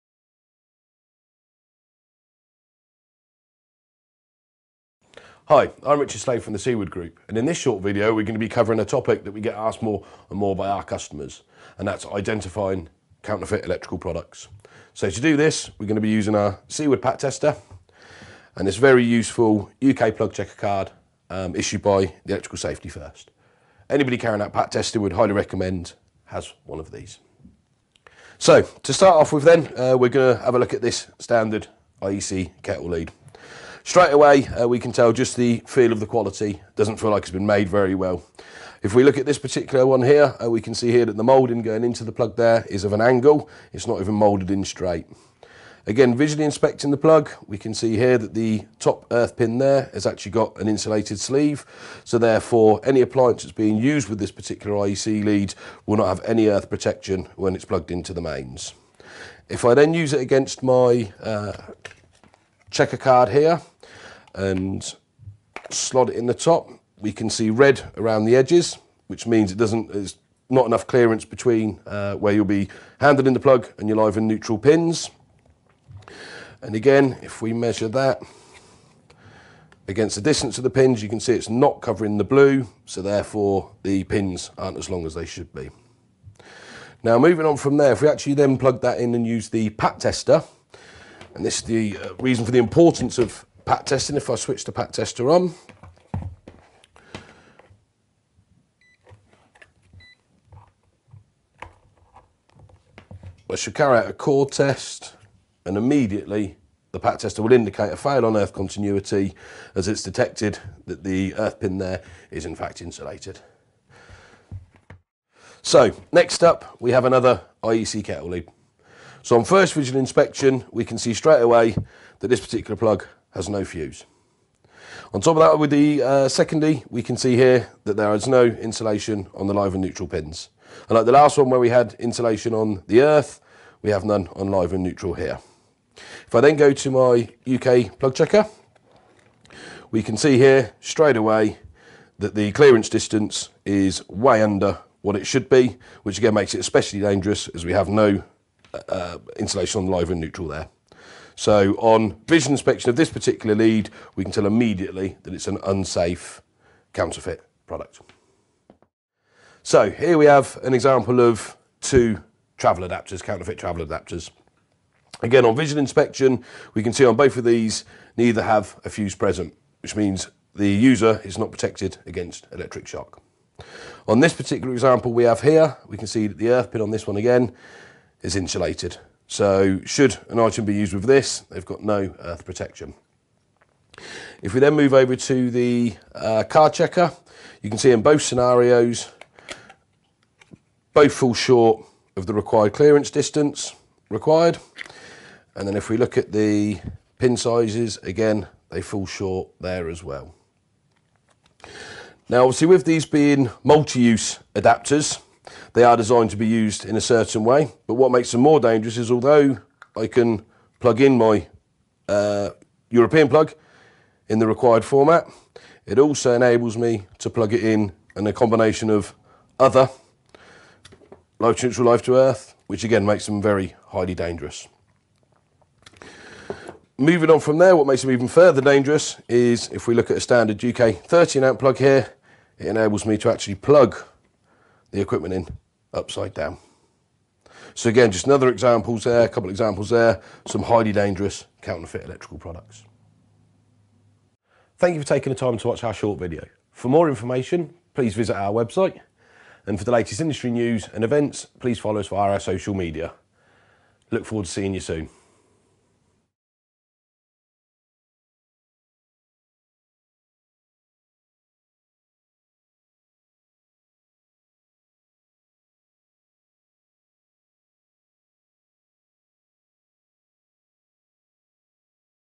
How to identify fake or counterfeit electrical products - Seaward
In this short video our PAT testing expert Richard Slade explains how to identify fake or counterfeit electrical products.

You can purchase a plug checker card from Electrical Safety First by visiting

to see our full range of PAT testers & kits

where you can ask him any PAT related questions.